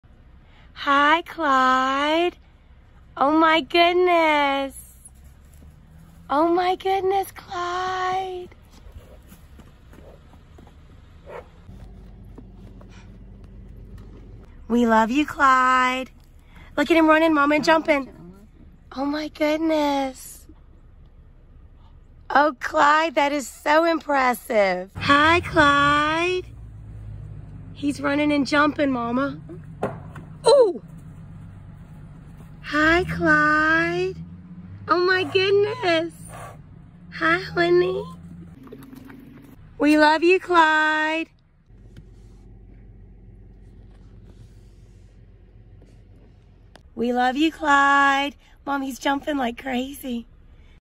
😮😍🥰 Adorable Mule Runs to See Toy Poodle Friend - Happy Dancing Mule //
Clyde the Mule runs to see Sebastian every time we visit him at Cedarock Park in NC! Look at those dance moves!

Video created with VITA